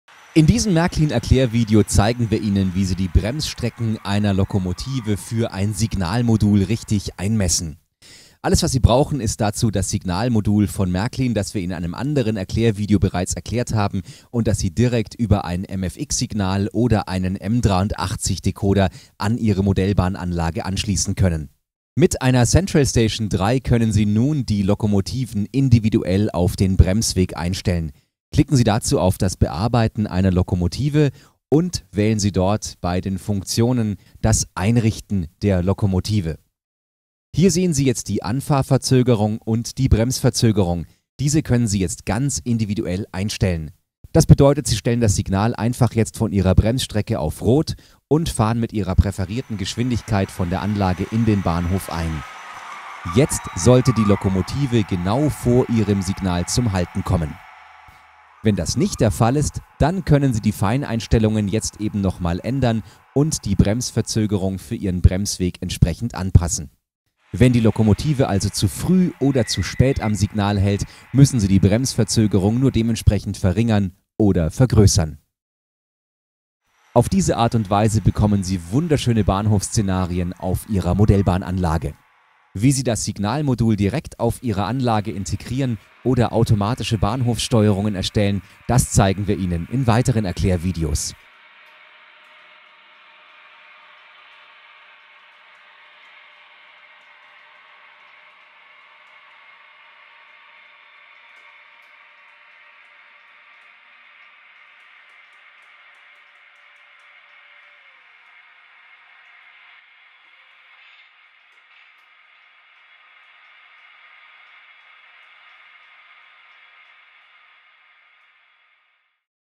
Lokomotiven für das Bremsmodul einmessen
In diesem Erklärvideo zeigen wir Ihnen, wie Sie Ihre Lokomotiven in der Central Station 3 für das Märklin Signalmodul einrichten, um ihre Züge realistisch langsam abzubremsen und langsam wieder anfahren zu lassen.